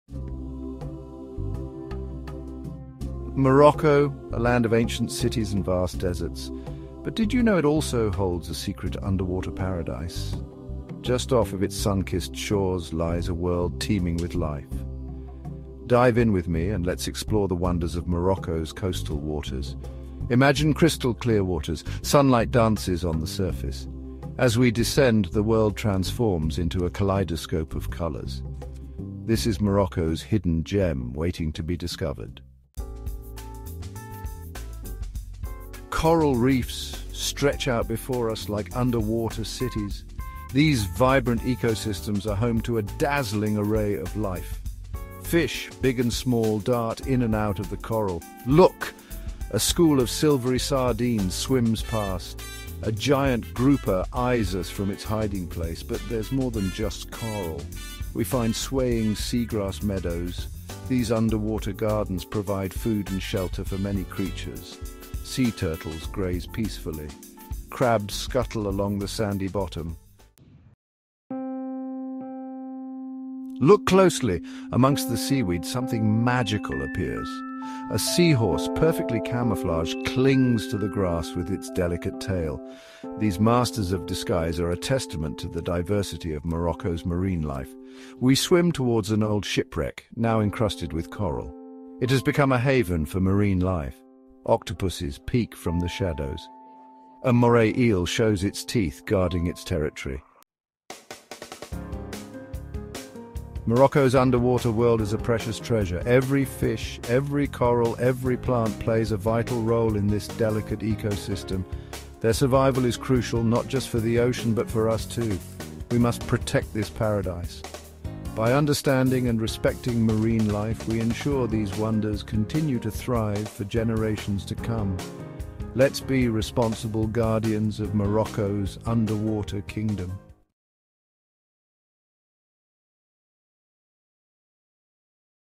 Morocco's Underwater Paradise: Exploring Marine Biodiversity!
Dive beneath the surface of Morocco’s coastal waters and uncover the stunning biodiversity that thrives in its vibrant coral reefs and seagrass meadows. These underwater ecosystems are teeming with life, from schools of colorful fish darting through the water to the delicate seahorses that hide among the seagrasses.

This underwater adventure takes you on a journey through the pristine waters, showcasing the rich variety of marine species, including octopuses, rays, and diverse invertebrates. The coral reefs, home to countless species of fish and marine life, provide essential habitat and serve as nurseries for young sea creatures.

However, these precious ecosystems face growing threats from climate change, pollution, and overfishing. Learn about the conservation efforts aimed at protecting these vital habitats and preserving the incredible species that depend on them. Get ready for an unforgettable look at the hidden world beneath Morocco’s coastal waters!